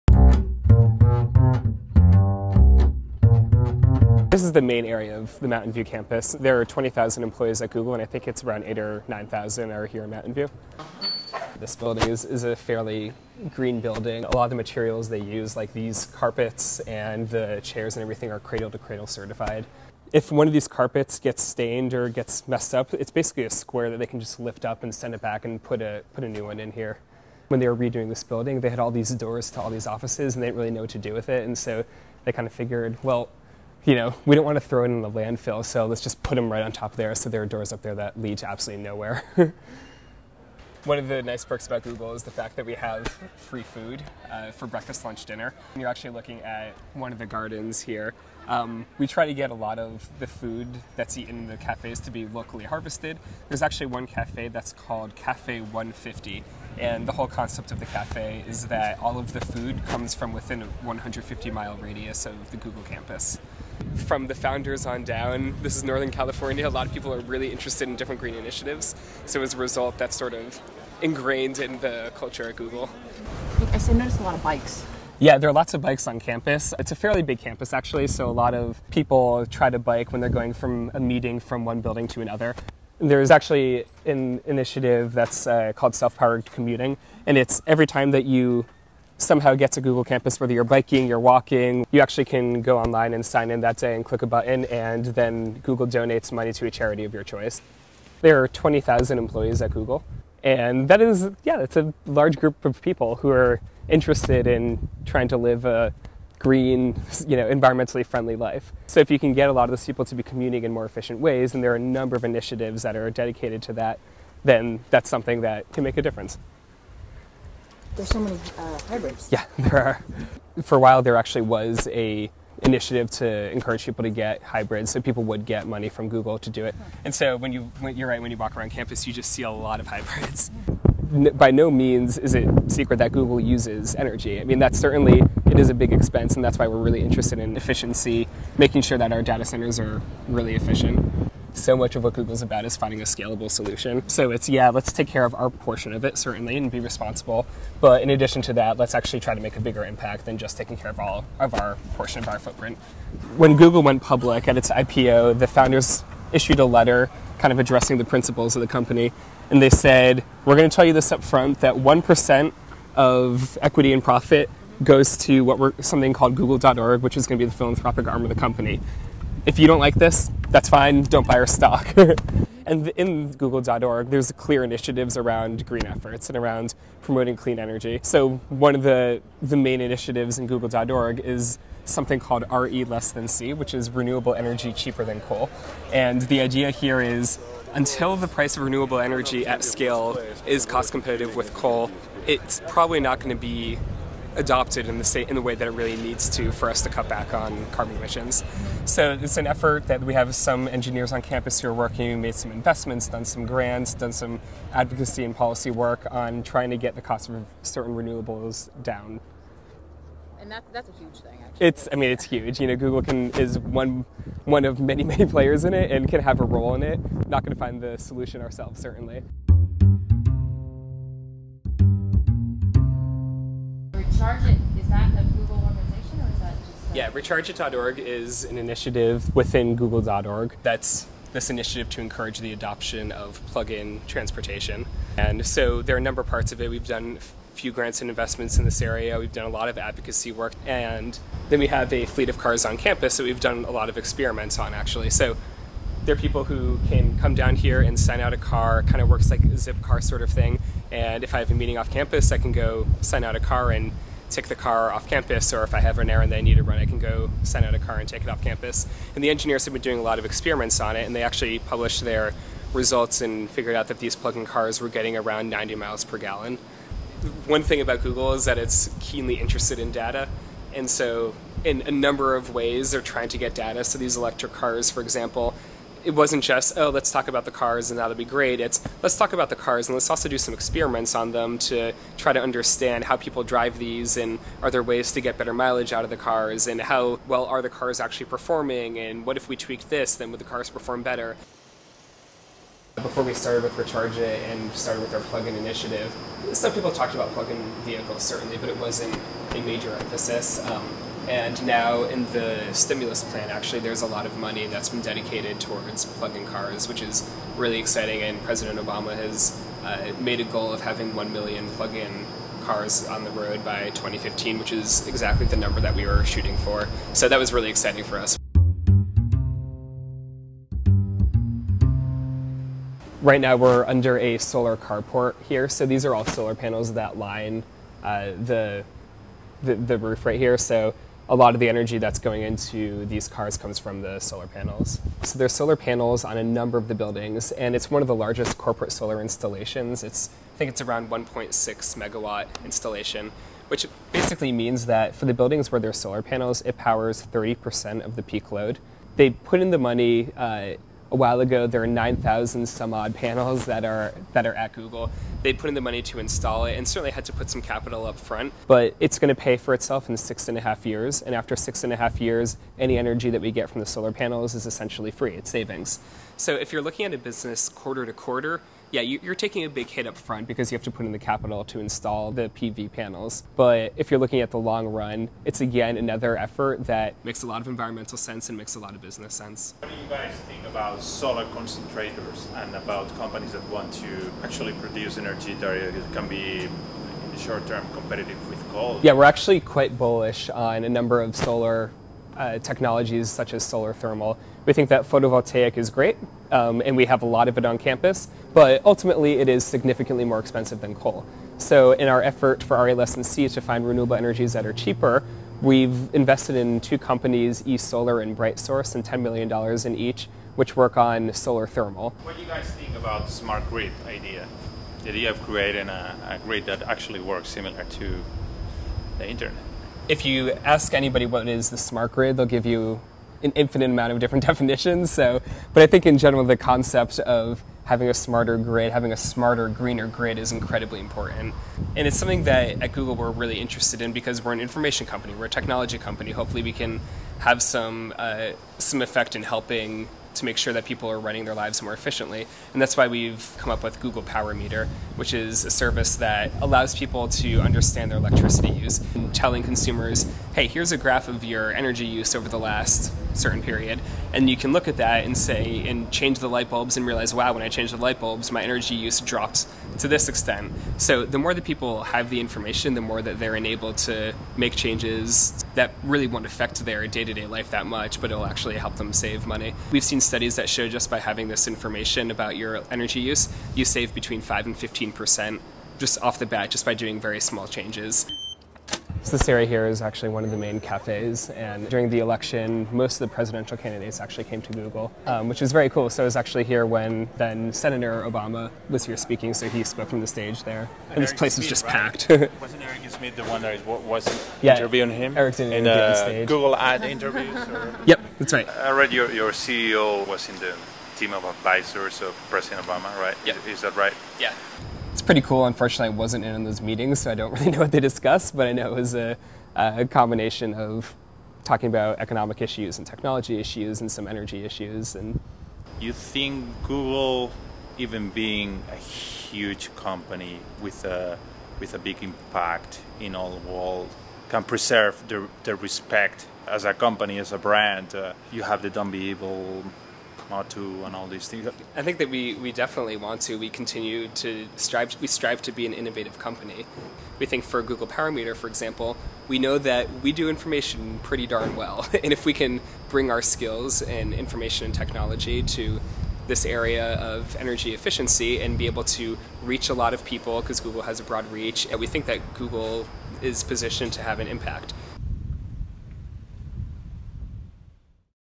Plug-ins and PV at Googleplex & the search for RE less than C
A tour of Google's HQ in Mountain View, CA to check out their plug-ins and photovoltaics, as well as garden, bikes and green buildings. Plus a chat about RE less than C.